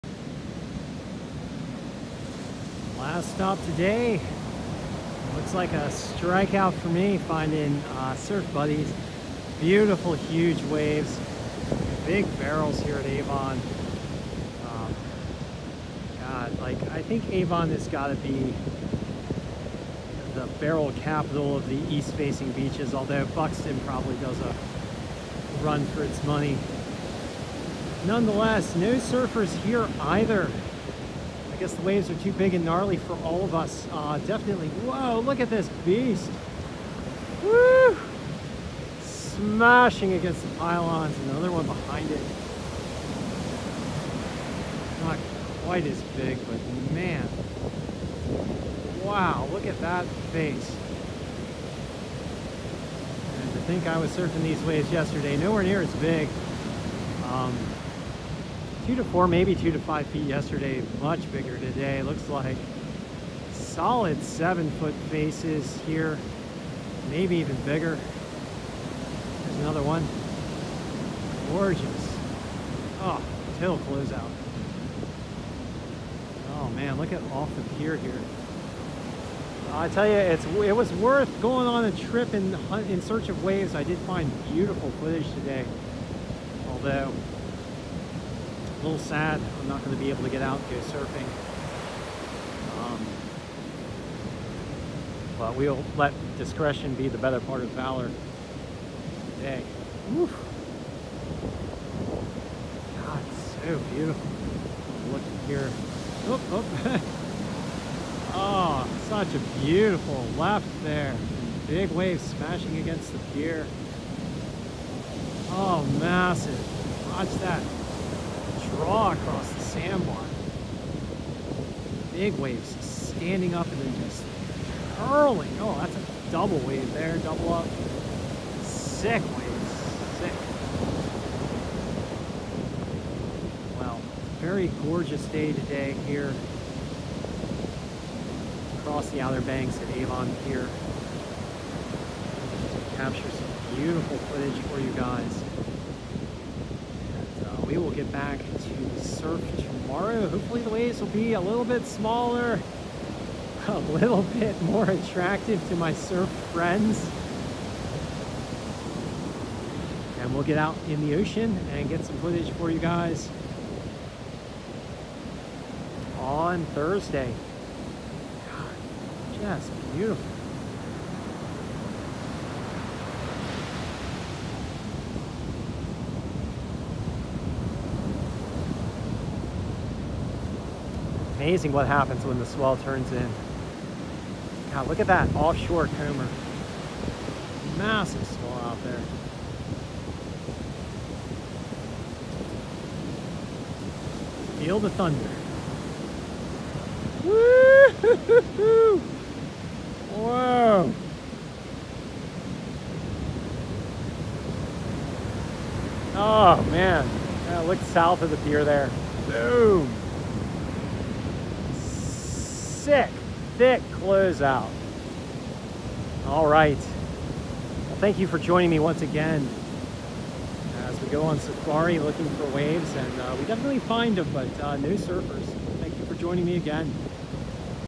Thunder at Avon
7 foot faces at Avon on March 1.